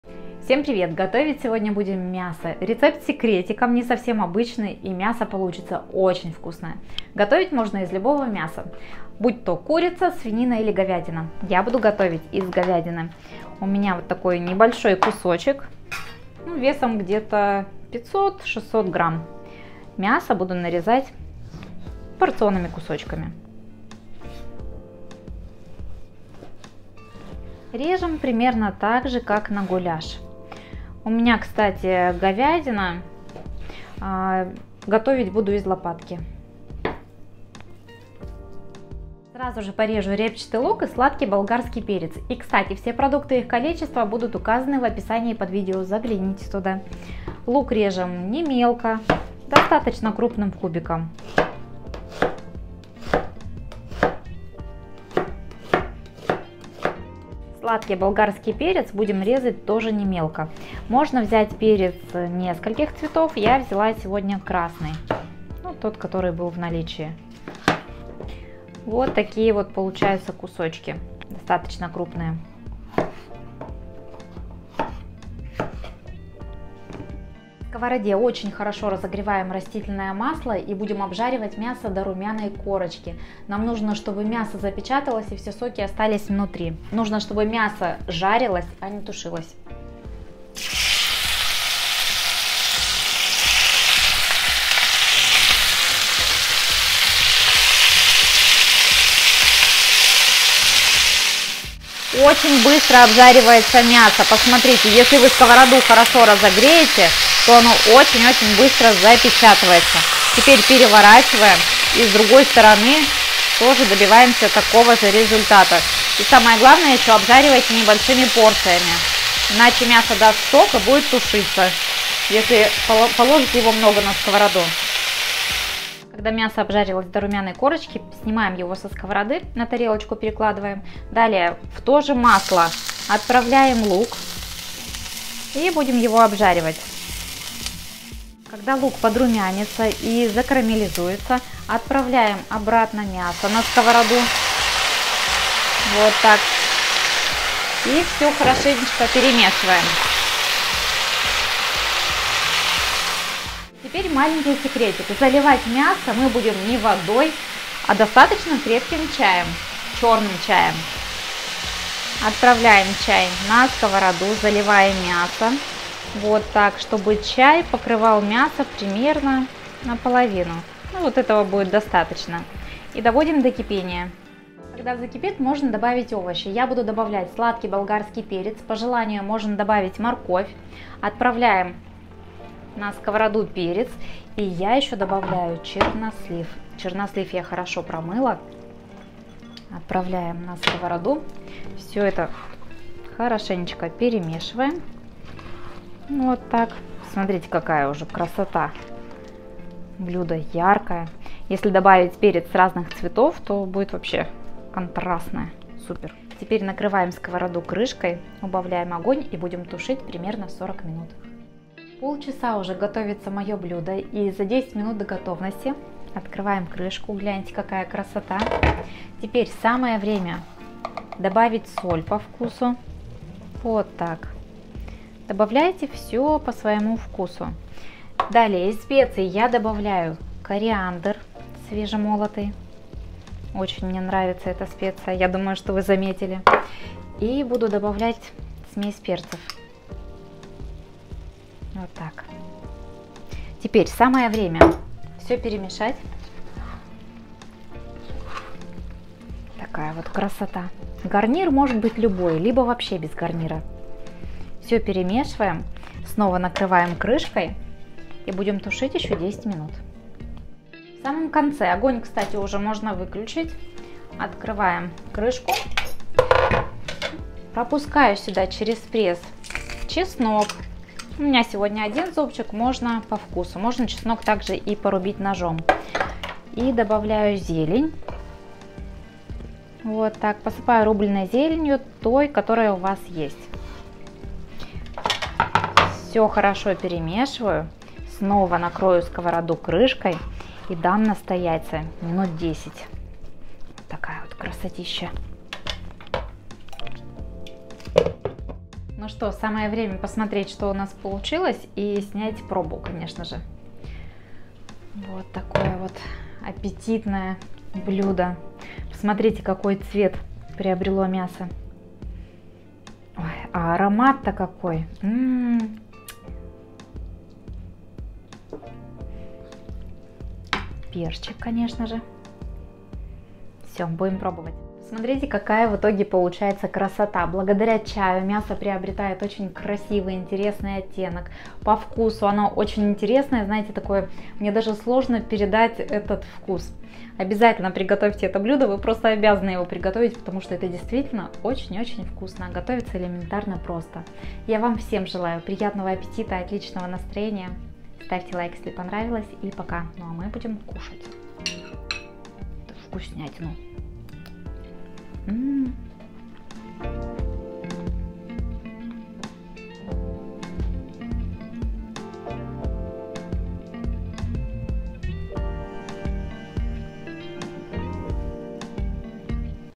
Вкусное МЯСО с секретиком! БОМБА рецепт! Рецепт простой,а МЯСО как в ресторане
Вкусное МЯСО с секретиком! БОМБА рецепт! Рецепт простой,а МЯСО как в ресторане,обязательно попробуйте приготовить по этому рецепту. Мясо вкусное,Как с гарниром,так и без него.

Присылайте Ваши фото блюд по нашим рецептам в ВК
ПРОДУКТЫ:
-мясо (говядина)-600гр
-лук-2-3шт
-перец болгарский-1шт
-растительное масло-для жарки
-чай  черный-300-мл
-чернослив-150гр
-соль-1ч.л.
-перец-1/2ч.л.
-кориандр-1/2ч.л.
-зелень-у меня кинза
-чеснок-2-3зубчика